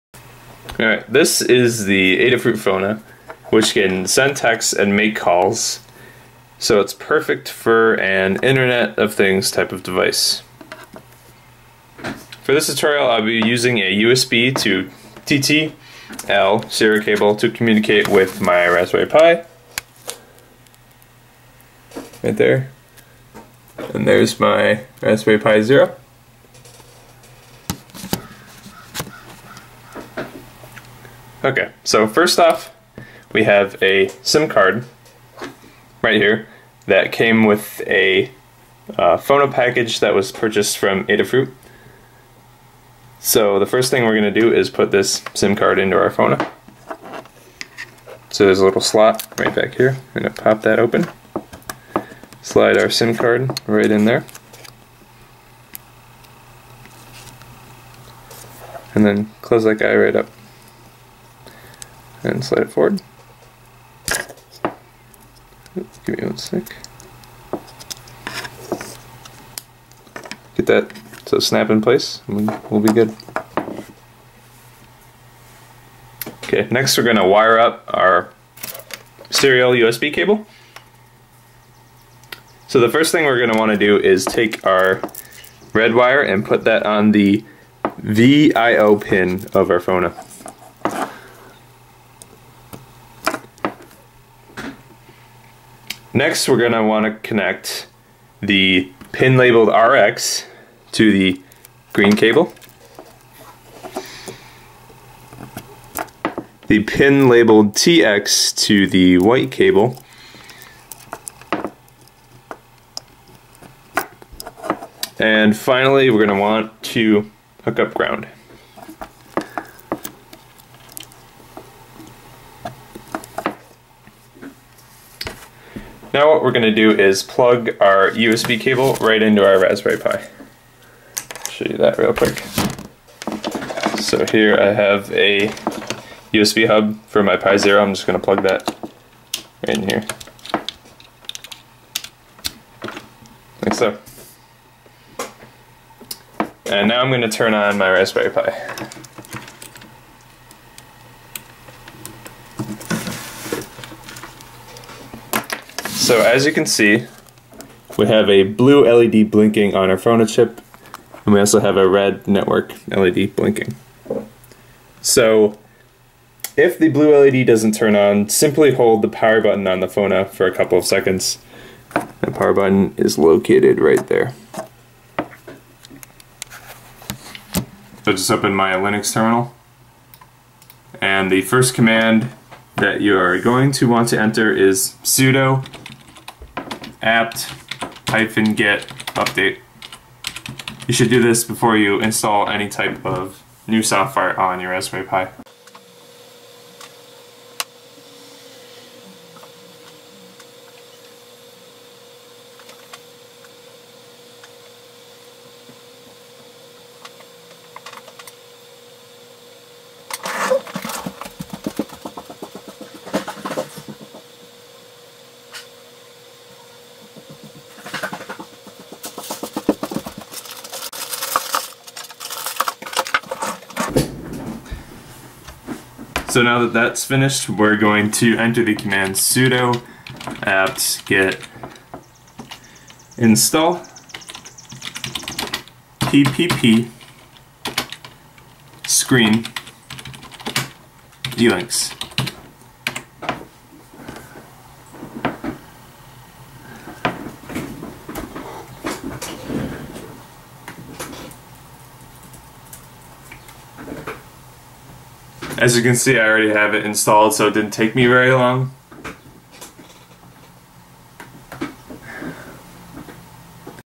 FONA Setup 1: Raspberry Pi IoT: Adafruit FONA Initial Configuration
Adafruit FONA setup for your Raspberry Pi! In this video I show you how to setup your Adafruit FONA, and use it with your Raspberry Pi and a USB serial cable.